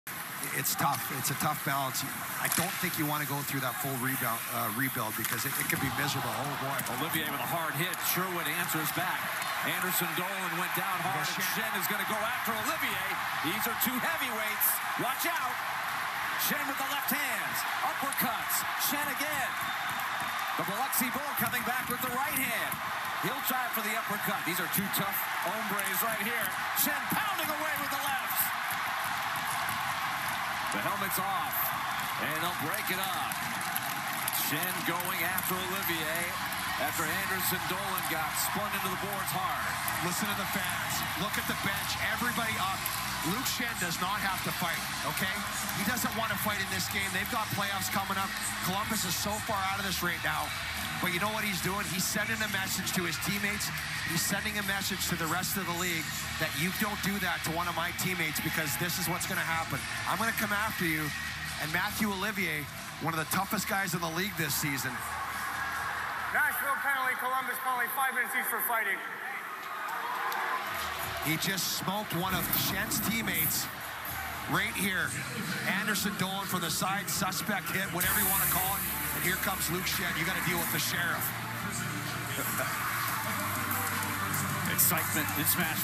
Mathieu Olivier vs Luke Schenn Apr 13, 2024
Mathieu Olivier vs Luke Schenn from the Columbus Blue Jackets at Nashville Predators game on Apr 13, 2024.